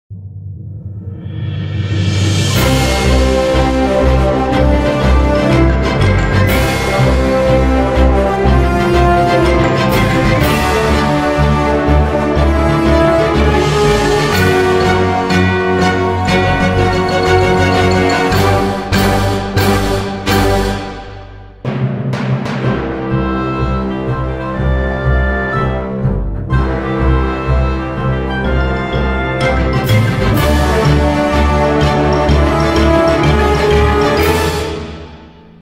nieuwe naam en Nieuwe Intro
ik vond kerkklokken NL te oud klinken en afgezien dat ik veel rollercoaster tycoon 3 speel heb ik het als volgt genoemd rollercoasterbells NL.